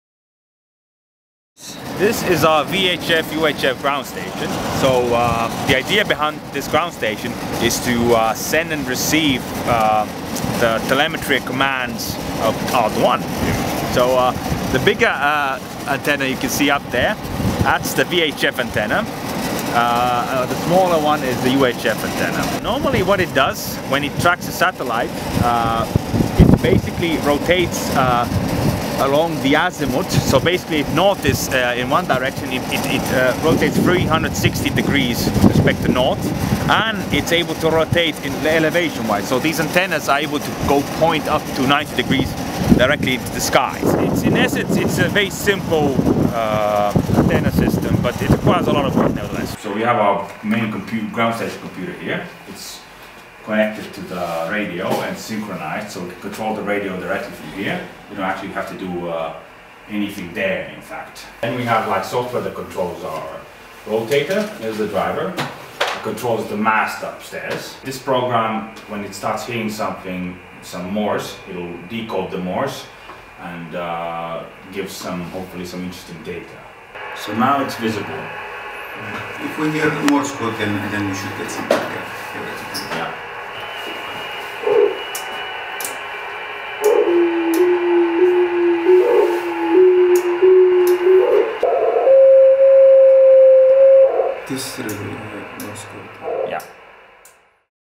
Aalto-1 // Quick Look on Ground Station
Systems engineer Antti Kestilä gives a brief introduction to our VHF/UHF ground station on the roof of ELEC building at Aalto University Otaniemi campus.